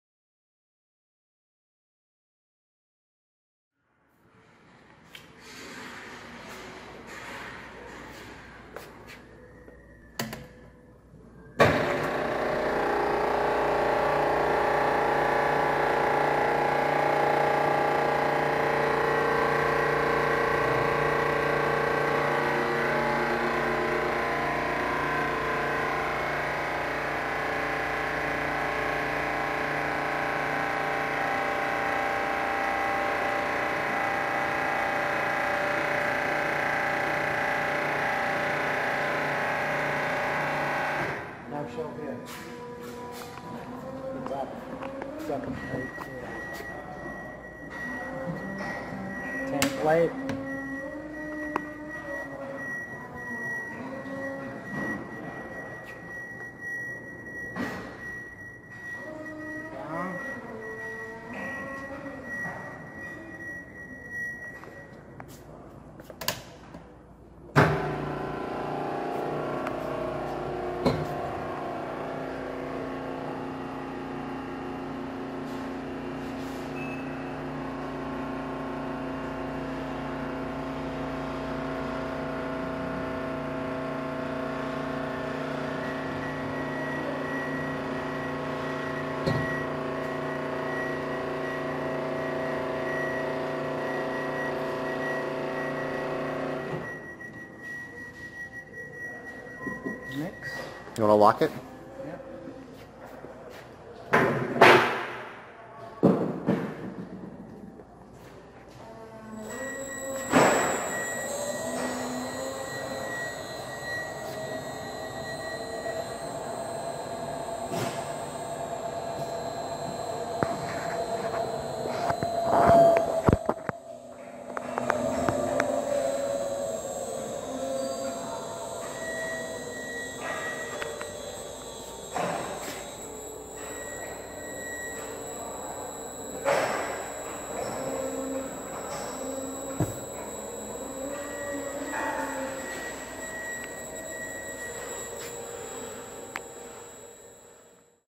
Olsa 850 Liter Mixing Skid
Olsa 850 Liter Mixing Skid - Listing 709665

EQUIPMENT DESCRIPTION
National Board Number: 59

Shell/Vessel:
MAWP: 324 KPa @ 156°C
MEXTP: 441 KPa @ 156°C
MDMT: -10°C @ 324 KPa

Jacket:
MAWP: 441 KPa @ 156°C
MDMT: -10°C @ 441 KPa
Control panel with automatic cover lifting and variable speed mixing
Allen Bradley Powerflex 70 5HP VFD for mixing control

TEST RESULTS: Customer reports that this homogenizer is a fully functional mix tank with automatic cover lifting and variable speed mixing. It has a custom built, wall mountable control panel. All necessary proximity sensor safeties are installed and functional. The tank and jacket is air tight and ready for a homogenizer pump and a vacuum pump if desired. The 4" butterfly valve on the bottom of the tank needs a new seal, (readily available)